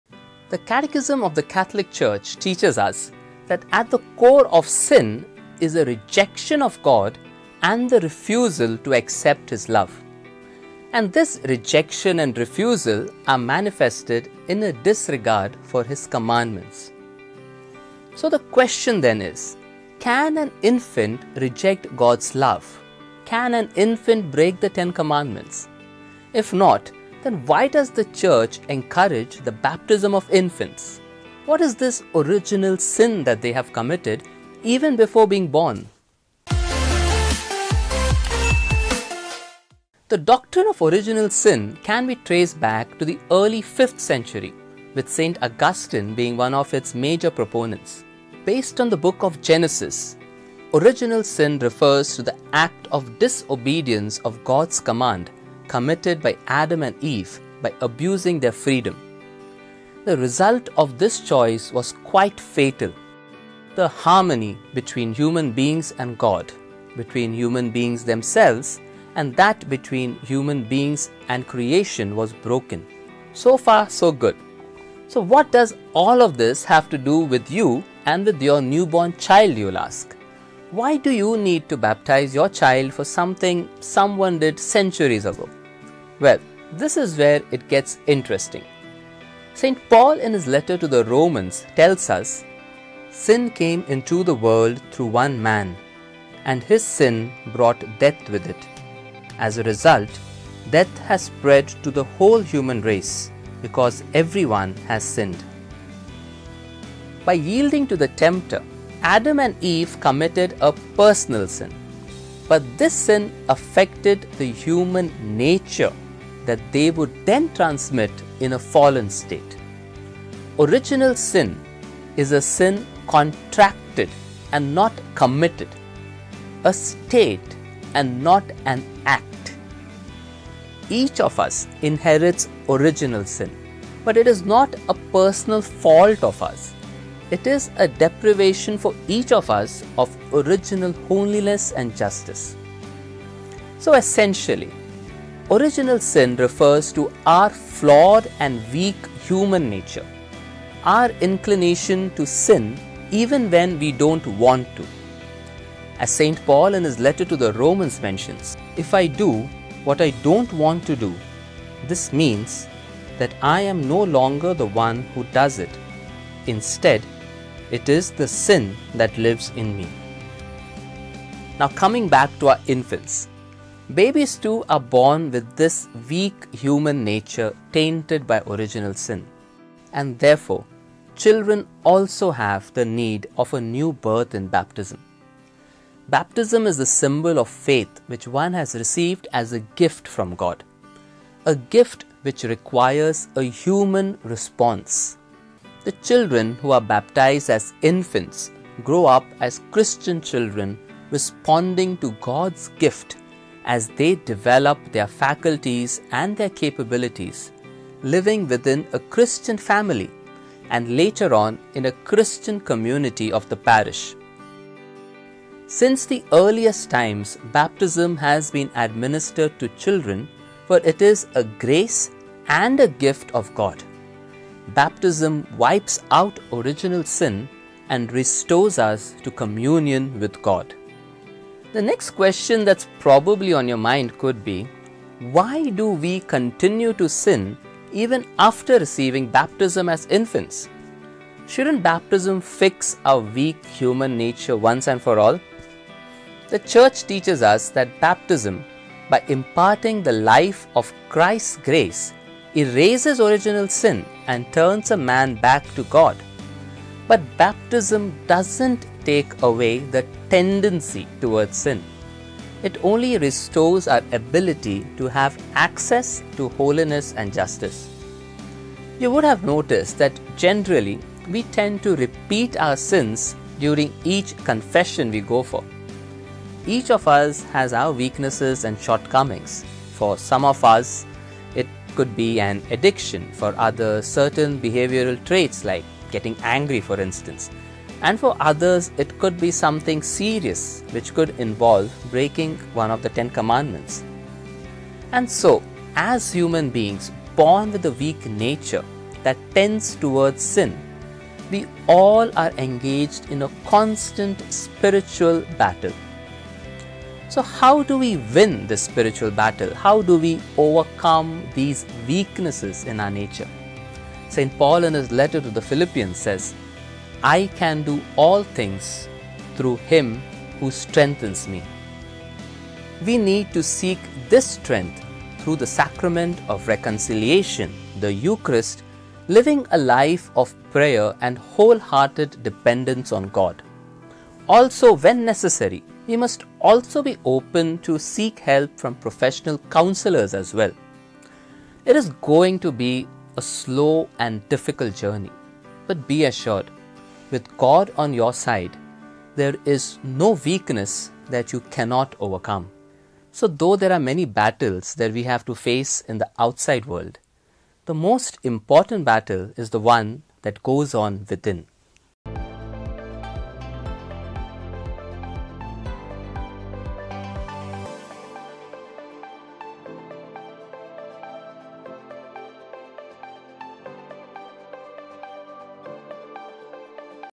Can Babies Sin? I Original Sin I Baptism
Have you ever wondered why innocent babies need to be baptized? Which sins have they committed? What is Original Sin exactly and how does it affect us all?  Watch this short video to better understand your own life.

DO YOU WANT TO KNOW MORE ABOUT THE JESUITS?